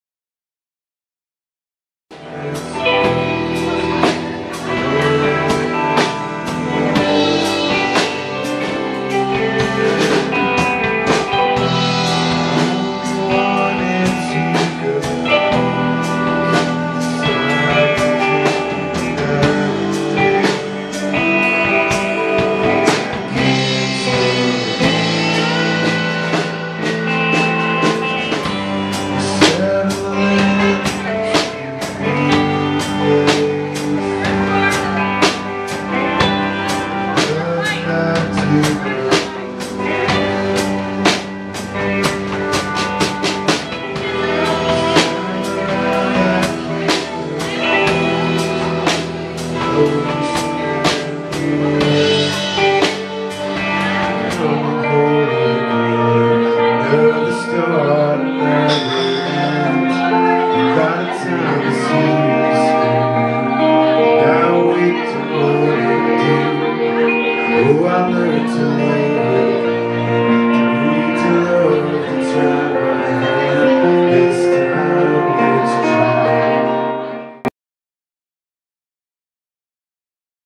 Mat Churchill & Backyard Town, Local Spins Wednesdays @ SpeakEZ Lounge (2/25/15)
Story at LocalSpins.com: Grand Rapids indie-folk band Mat Churchill & The Backyard Town perform at SpeakEZ Lounge in Grand Rapids as part of the Local Spins Wednesdays series.